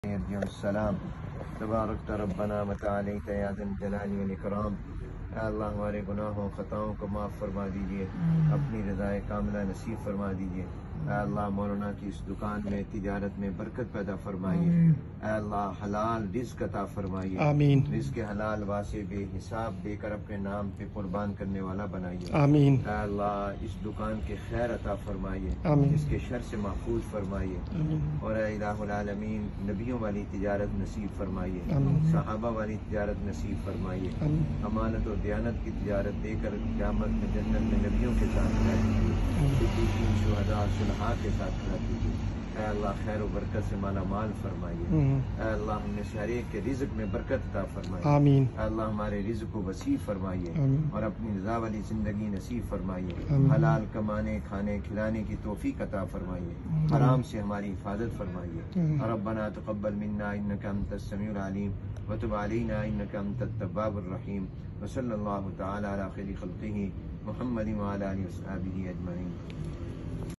Dua by Qari Muhammad Hanif Jalandhari at Momaya Fragrance
Shaikh Hanif visited Momaya Fragrance